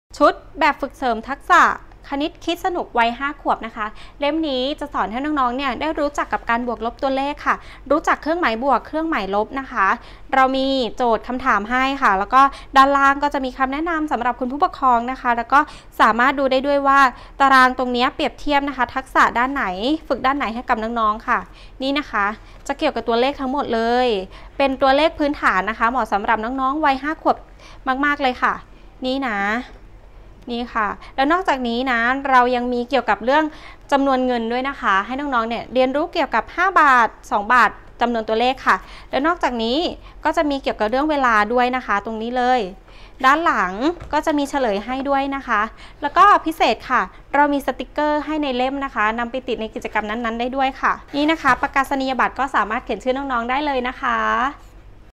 หนังสือชุด แบบฝึกเสริมทักษะ 5 ปี คณิตคิดสนุก
ใช้หลักการและเหตุผลในการวิเคราะห์หาคำตอบโจทย์ปัญหาทางคณิตศาสตร์ได้
ฝึกให้เด็กๆ คุ้ยเคยกับการคิดแล ะตั้งคำถามจะช่วยเพิ่มความมั่นใจให้กับเขา
สำหรับเด็กๆ บางคนแล้ว วิชาคณิตศาสตร์ถือว่าเป็นฝันร้ายสำหรับพวกเขา นั่นอาจเป็นเพราะได้รับการส่งเสริมที่ผิดวิธี ด้วยความเข้าใจผิดที่ว่าถ้าเด็กๆ มีทักษะการคำนวณดี ก็จะเก่งวิชาคณิตศาสตร์ จึงให้เด็กๆ ฝึกฝนการคำนวณซ้ำไปซ้ำมา จนทำให้พวกเขาเกิดความเบื่อหน่ายและไม่สนใจวิชาคณิตศาสตร์ในที่สุด

วิชาคณิตศาสตร์เป็นวิชาที่ต้องอาศัยการสะสมและการเชื่อมโยงความรู้ ดังนั้นการพัฒนาความรู้ด้านคณิตศาสตร์ของเด็กๆ จึงจำเป็นต้องวางแผนให้ดี เมื่อเด็กๆ อยู่ในวัยที่พร้อมจะเรียนรู้แล้ว พวกเขาจะต้องหมั่นทำแบบฝึกหัดที่จะช่วยส่งเสริมความสามารถในการแก้ไขปัญหา รู้จักเชื่อมโยง และคิดวิเคราะห์ แก้ปัญหาได้ซึ่งเด็กที่มีพื้นฐานทางคณิตศาสตร์ที่ดีจะมีแนวโน้มเป็นคนที่รู้จักแสดงความคิดเห็น คิดวิเคราะห์ และจัดการกับปัญหาได้ดีในอนาคต
* Website สำนักพิมพ์เอ็มไอเอส

💙 Facebook เลี้ยงลูกด้วยหนังสือ
💛 Line @Kidbook
💚 Shopee ร้านสำนักพิมพ์เอ็มไอเอส
💜 Lazada ร้านสำนักพิมพ์เอ็มไอเอส